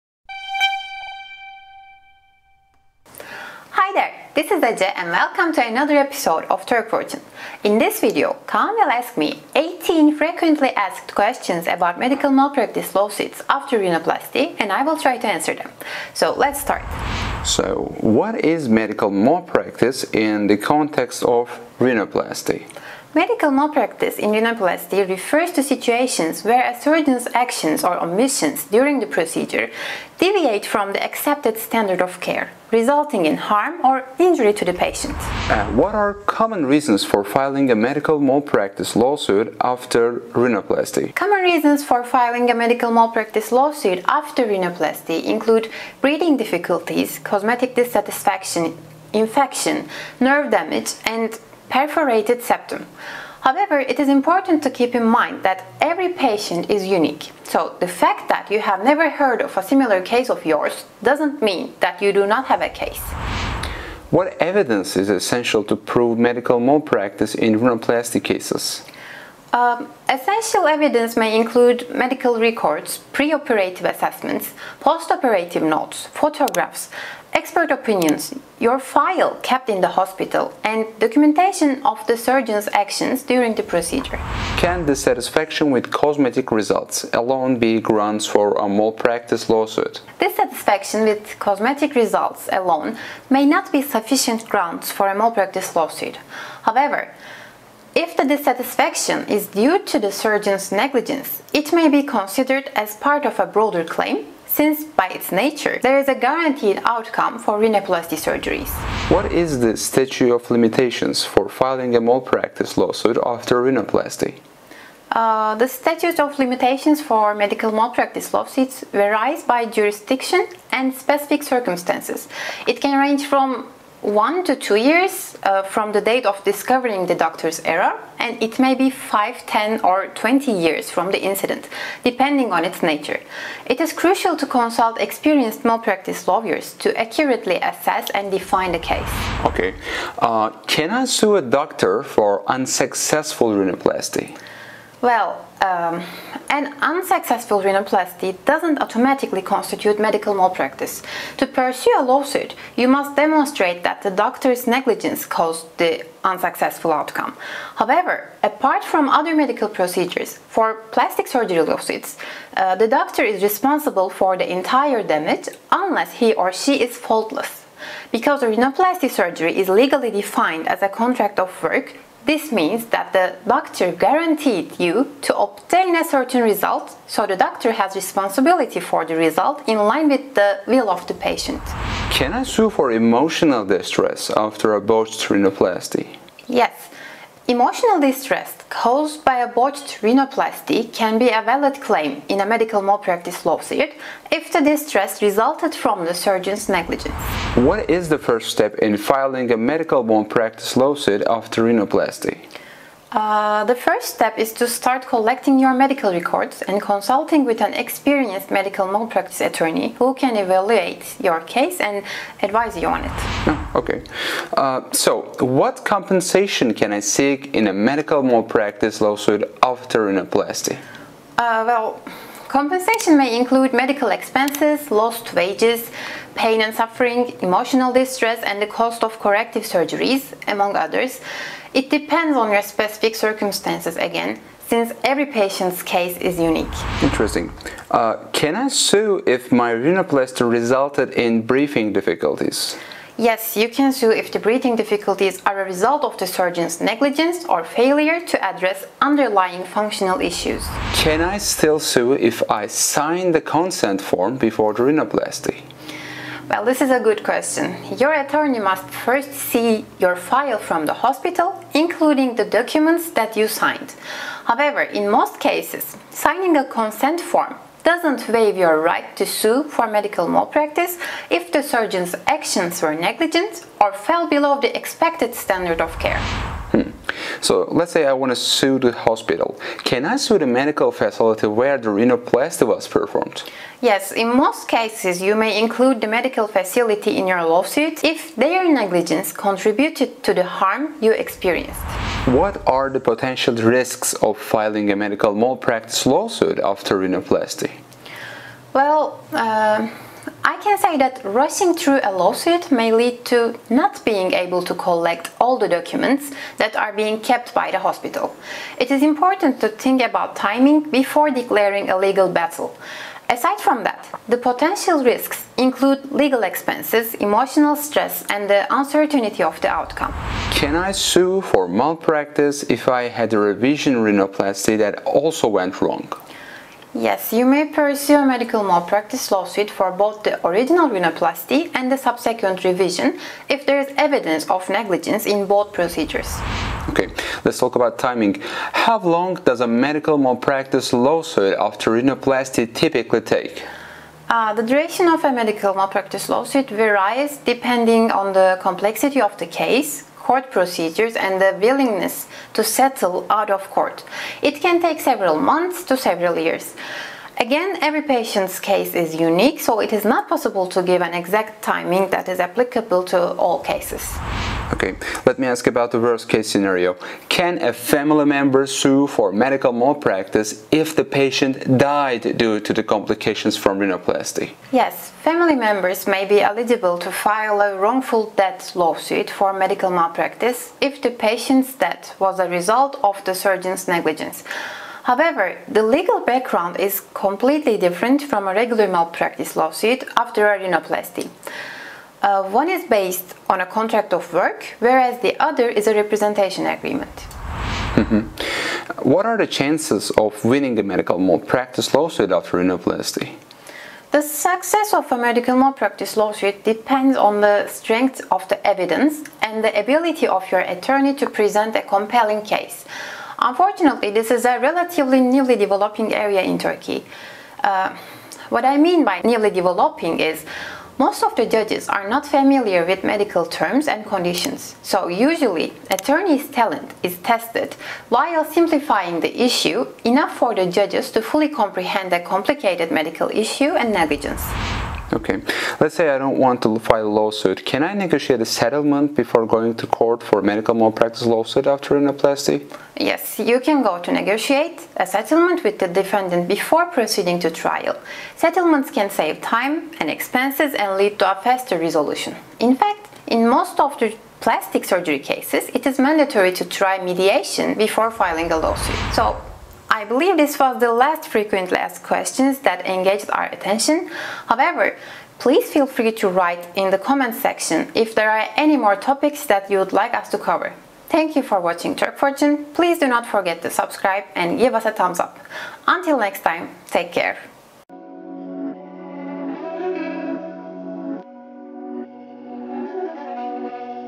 18 FAQs on Botched Nose Jobs: Medical Malpractice Expertise in Turkey
In this video, medical malpractice lawyer Ece Özlü Beylen sheds light to 18 frequently asked questions regarding lawsuits arising from rhinoplasty failures in Turkey. These 18 questions are the ones we receive most from people who experienced a failure after getting a nose job in Turkey. We wish this video helps you to learn more about this topic if you are in this unfortunate situation.

Turkey Pays You to Bank Here! - FX Protected Yuvam Accounts of Turkey

00:00 Intro
00:16 1. What is Medical Malpractice in Rhinoplasty?
00:35 2. Reasons to File a Lawsuit after Rhinoplasty
01:03 3. Evidences of Rhinoplasty Failure
01:24 4. Lawsuit Based on Cosmetic Dissatisfaction
01:49 5. Deadline for Lawsuit after Surgery
02:20 6. Lawsuit Based on Unsuccesful Rhinoplasty
03:02 7. Lawsuit Based on Emotional Distress
03:17 8. First Step of the Procedure
03:34 9. Contents of the Compensation Claim
03:57 10. Lawsuit based on Breathing Difficulties
04:11 11. Signing a Consent Letter Before Surgery
04:36 12. Can I Sue the Hospital?
04:52 13. Risks of Filing a Malpractice Lawsuit
05:21 14. Lawsuit after Revision Rhinoplasty
05:37 15. How Long the Laswuit Takes?
06:06 16. Lawsuit After Death Due to Malpractice
06:40 17. Chances of Winning a Malpractice Lawsuit
07:15 18. Settlement with the Doctor before Lawsuit
07:40 Contact Us